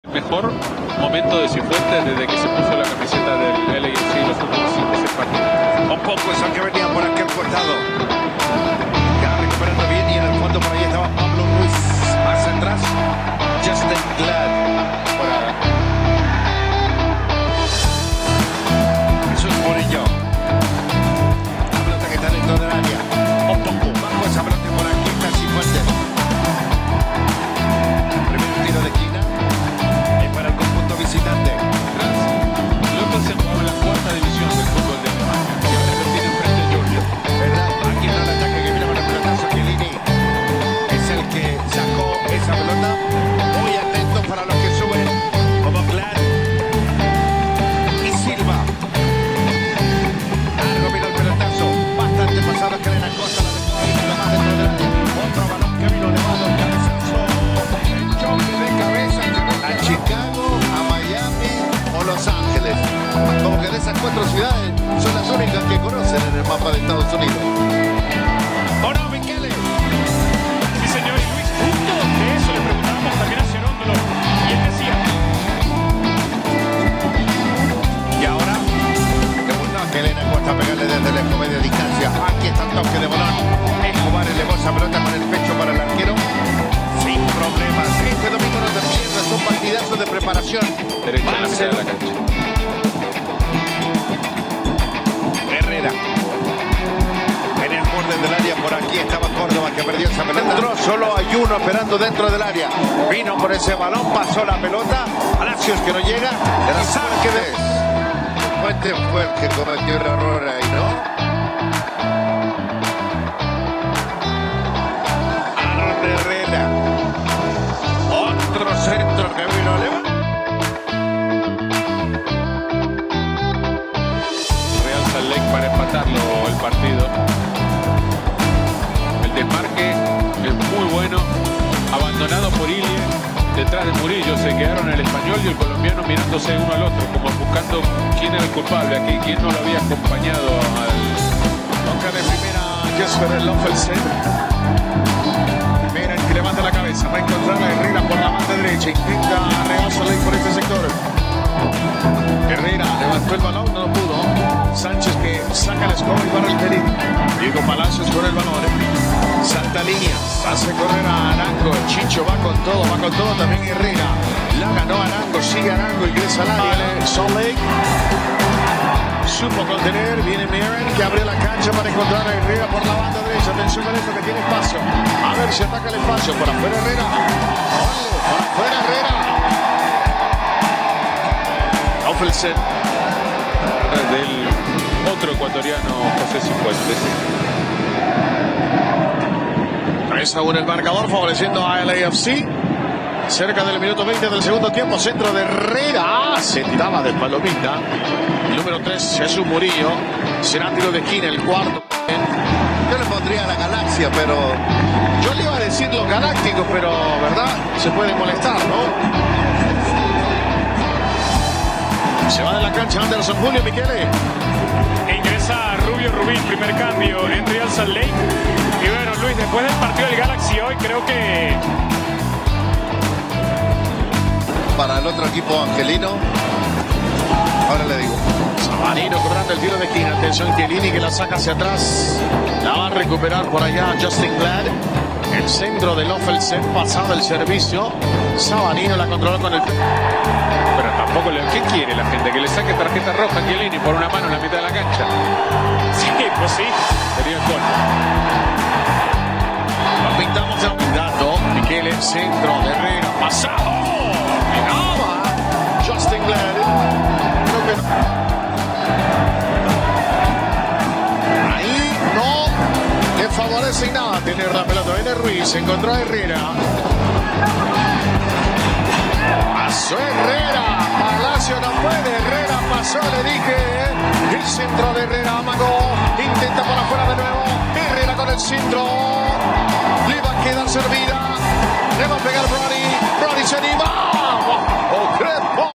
Resumen Aaron Herrera y Rubio rubín en RSL vs Los ángeles fc
SALUDOS... 💯💯🇬🇹👍
Yo solo edito las jugadas de los dos guatemaltecos su participación en el partido.

Los mejores momentos de los dos legionarios guatemaltecos en Real Salt Lake De MLS.

Mil disculpas por la calidad de video...

Derechos de video 100 %:
De MLS y TUDN

CRÉDITOS DE MÚSICA DE FONDO;